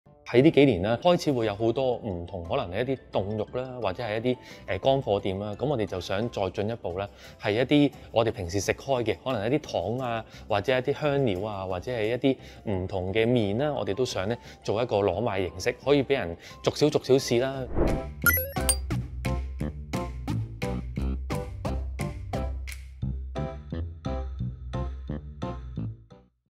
【創業ideas】裸買食品吸客有方 3招抗疫兼宣傅 今日13:30 準時收睇
港人何凱傑9年前開設綠色概念雜貨店「Pimary」，售賣標榜公平貿易和本地製作的產品。去年12月他再增設裸買雜貨店，主張客人自備容器，購買無包裝食品，從源頭開始減廢。即睇有咩招數吸客兼宣傳啦!